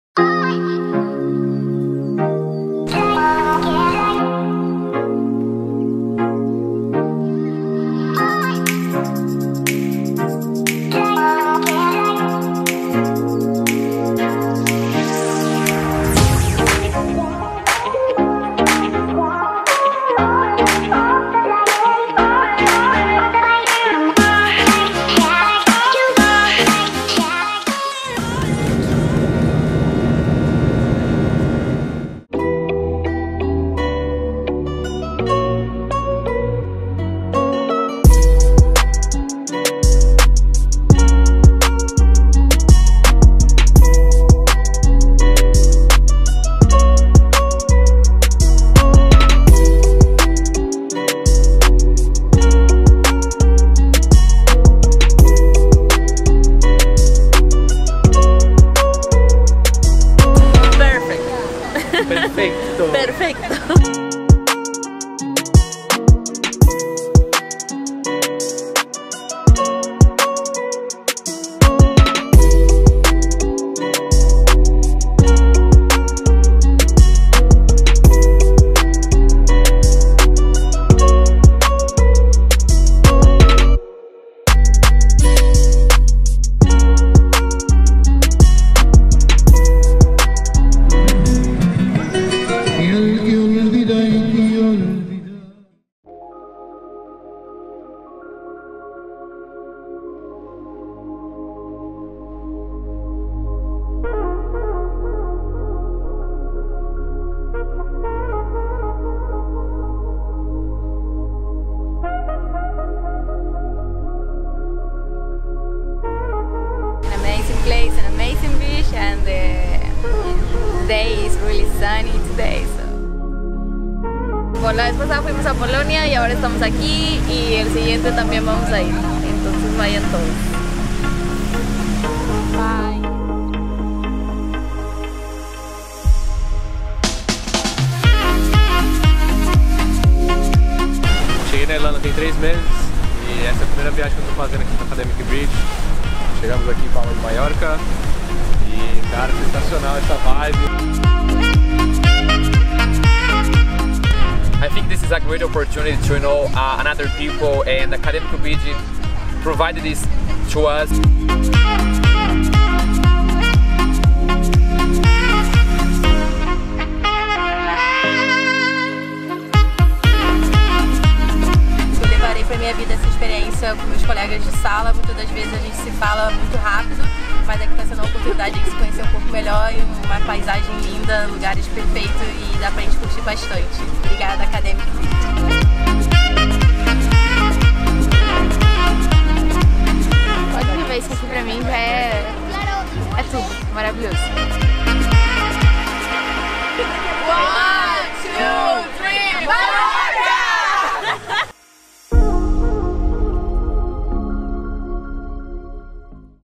Trip to Palma de Mallorca 2019 l Academic Bridge
Palma de Mallorca, thank you for having us! It was the best and most memorable way to say goodbye to the summer! Check out our video and see you at our next destination!